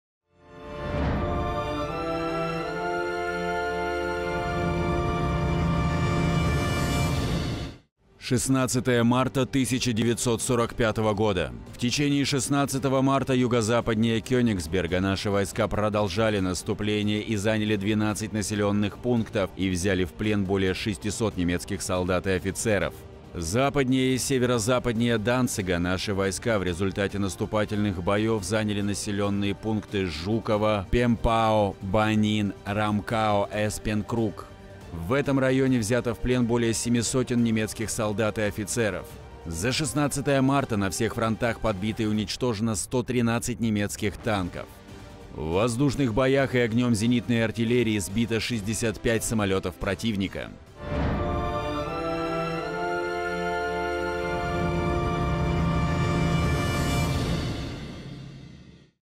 Военные хроники. 16 марта 1945 года. До Победы - 55 дней
В течение 16 марта юго-западнее Кенигсберга наши войска продолжали наступление и заняли 12 населённых пунктов и взяли в плен более 600 немецких солдат и офицеров.
Западнее и северо-западнее Данцига наши войска в результате наступательных боёв заняли населённые пункты Жуково, Пемпау, Банин, Рамкау, Эспенкруг. В этом районе взято в плен более 700 немецких солдат и офицеров.
За 16 марта на всех фронтах подбито и уничтожено 113 немецких танков. В воздушных боях и огнём зенитной артиллерии сбито 65 самолётов противника.